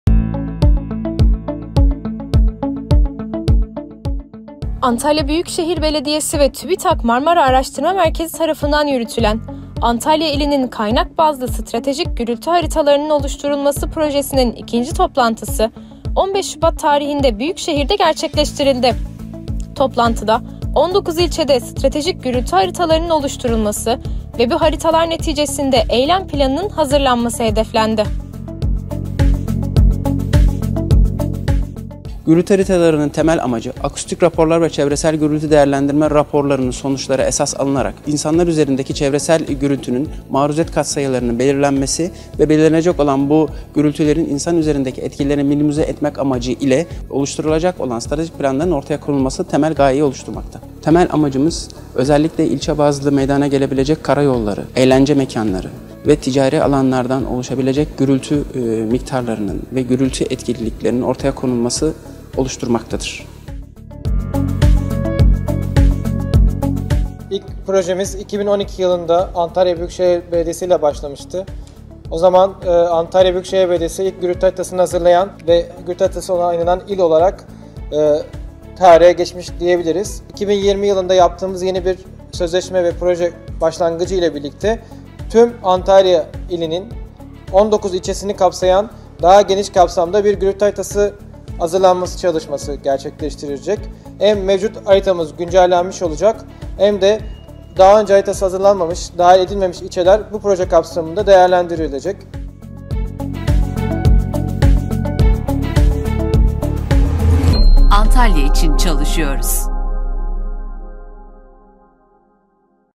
Çevreci Toplantımızı Gerçekleştirdik | Antalya
TÜBİTAK Marmara Araştırma Merkezi ile yürüttüğümüz 'Antalya İlinin Kaynak Bazlı Stratejik Gürültü Haritasının Oluşturulması’ Projesi'nin 2'nci toplantısını gerçekleştirdik.

Oynatma Listeleri;